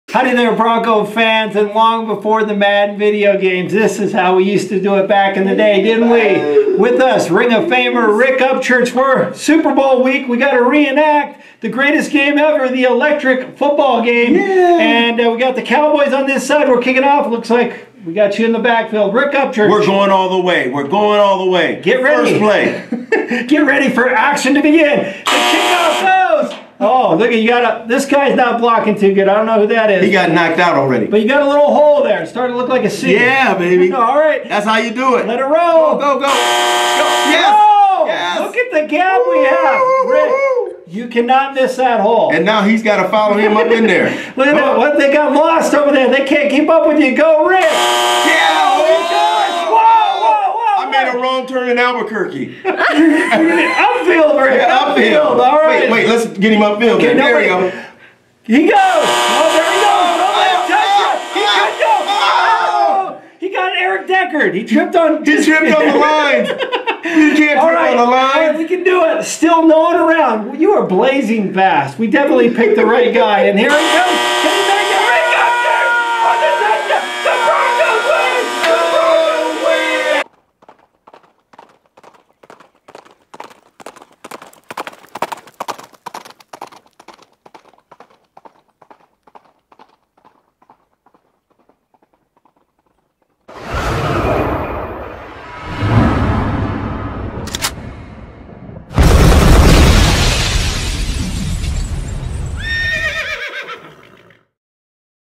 Electric Football with Rick Upchurch
Going old school. The greatest football game ever with one of the best returners in NFL history, Rick Upchurch.